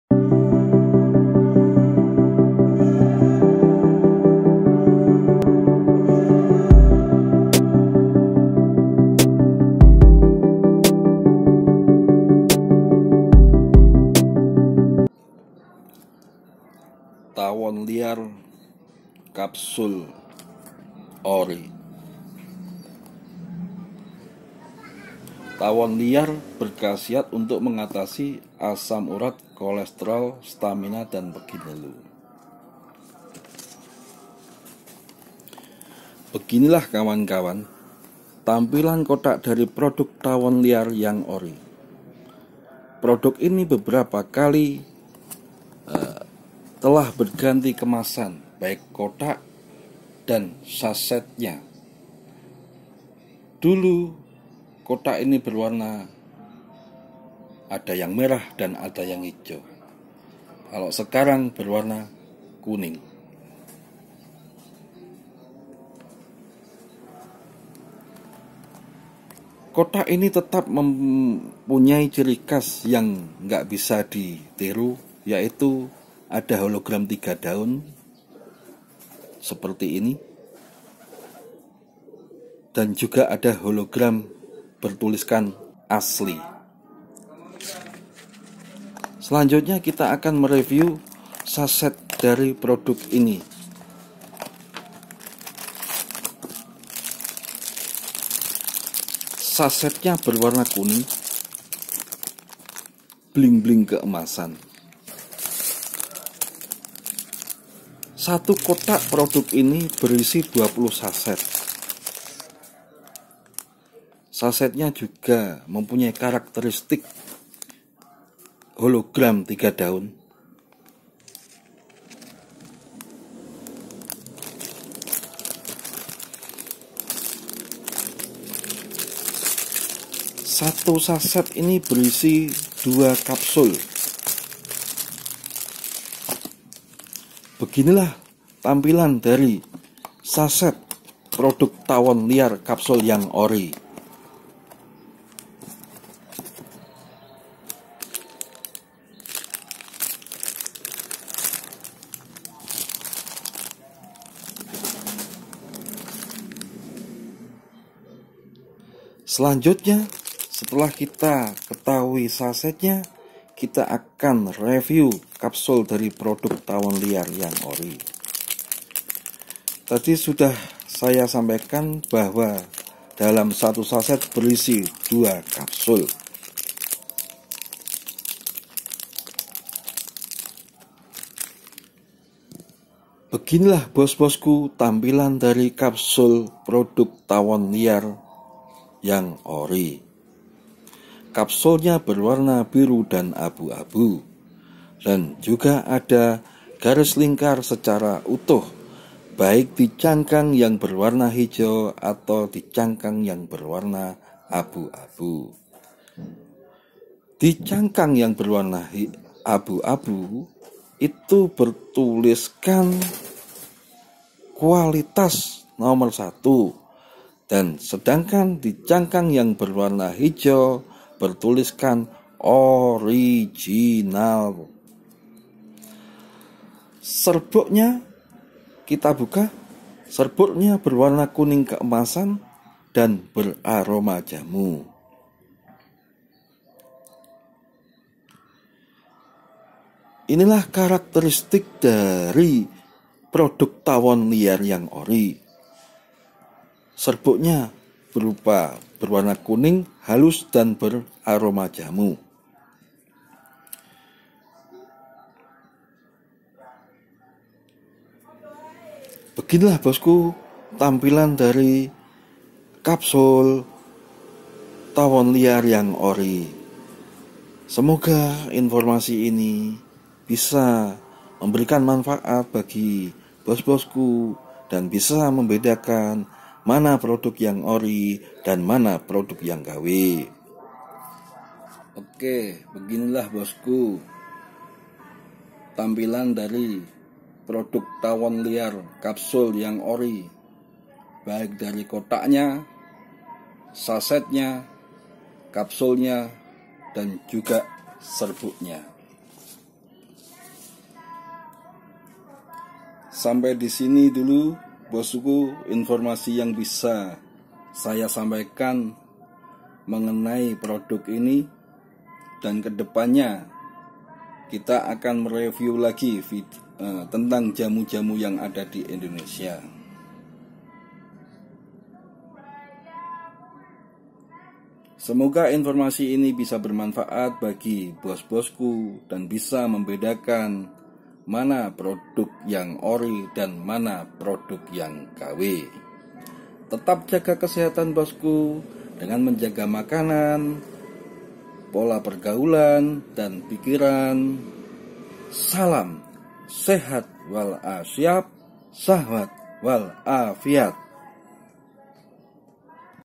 Tawon Liar Kapsul Ori Obat Asam Urat, Kolesterol & Pegel Linu. 1000% ORI!!!
Tawon Liar berkhasiat mengatasi Asam Urat, Kolesterol, Pegel Linu.
Tawon Liar merupakan produk jamu yang dikemas berbentuk kapsul.
Komposisi :
Curcumae Rhizoma
Zingiberies Rhizoma
Zingiberies Aromaticae
Panax Ginseng
Royal Jelly
Cara Konsumsi :
Minum 2 kapsul bilamana perlu. Bila parah, boleh minum 2x2 kapsul/hari.